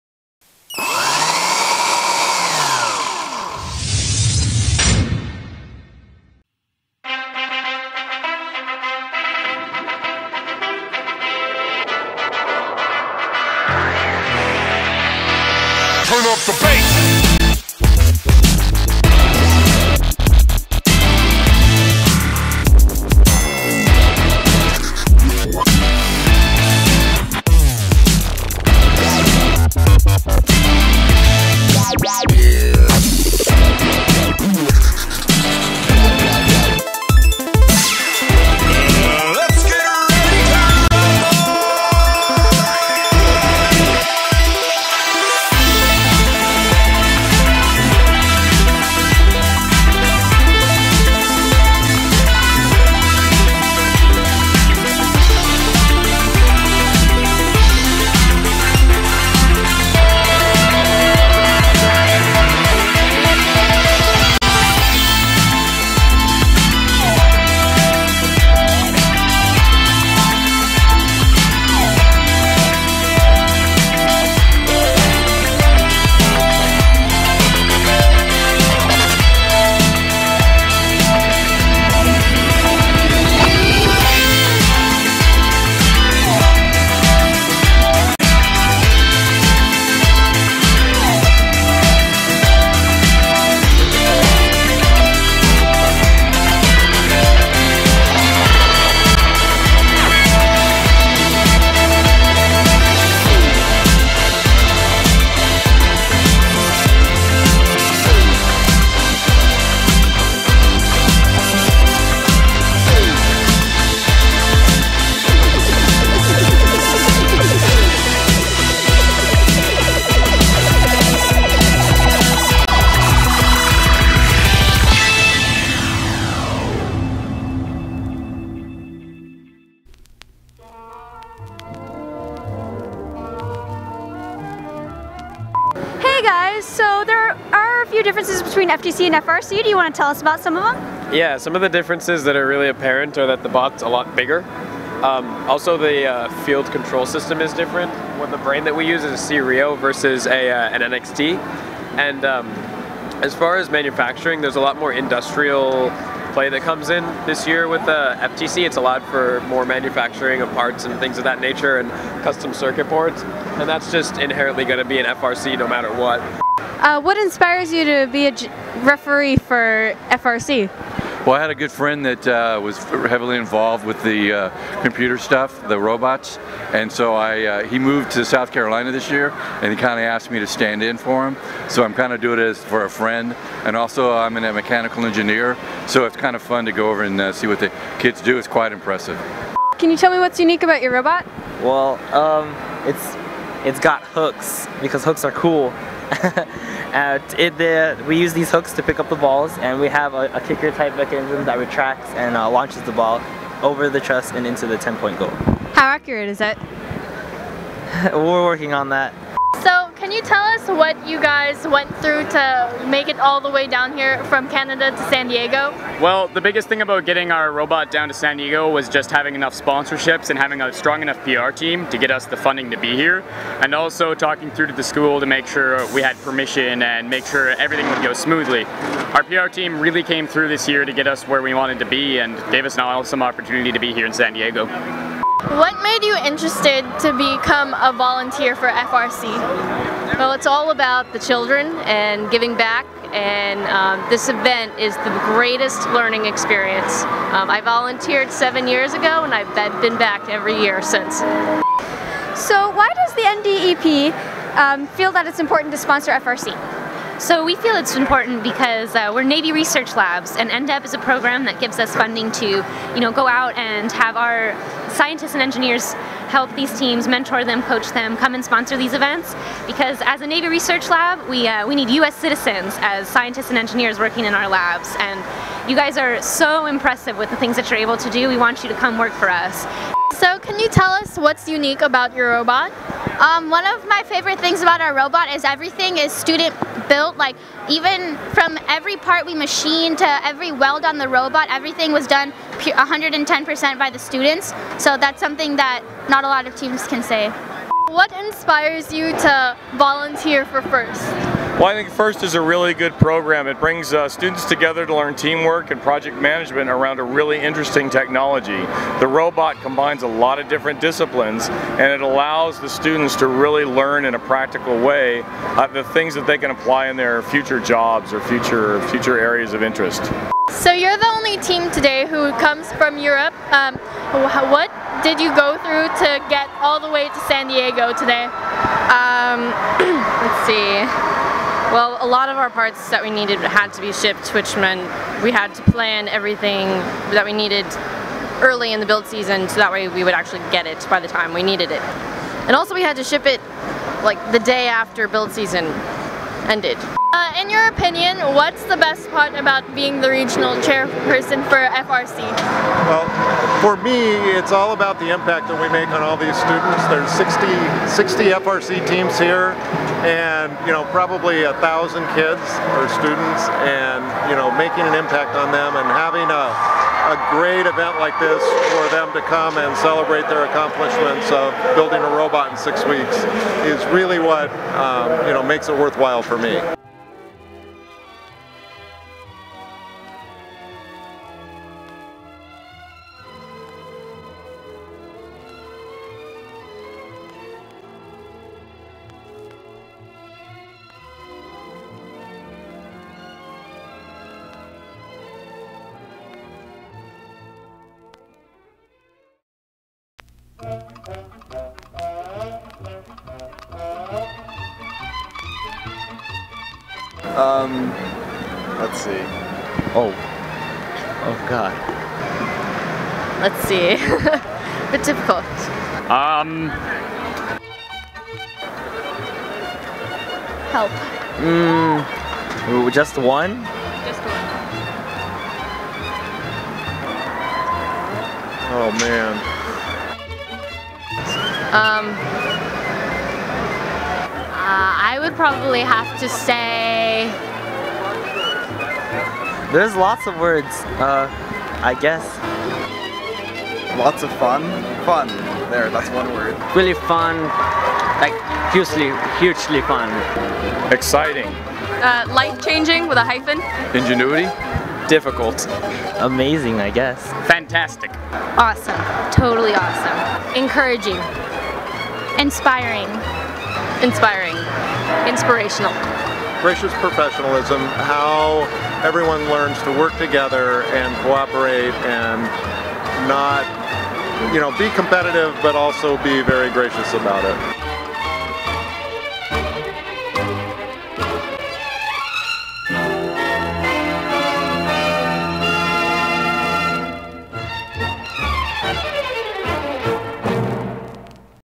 2014 San Diego Regional FRC Championship
March 8, 2014 was the final day of the San Diego FIRST Robotics Competition Championship. The Stuffed Dragons were there to capture the fun!